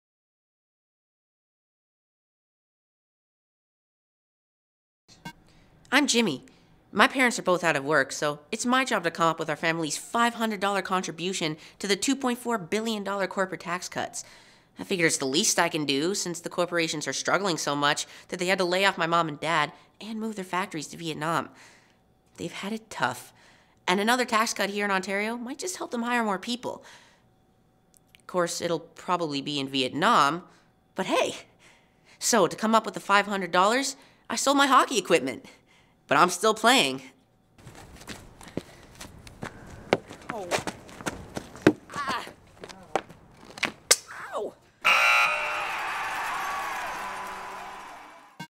Sold His Hockey Equipment
People For Corporate Tax Cuts member Jimmy sold his hockey gear to help pay for corporate tax cuts!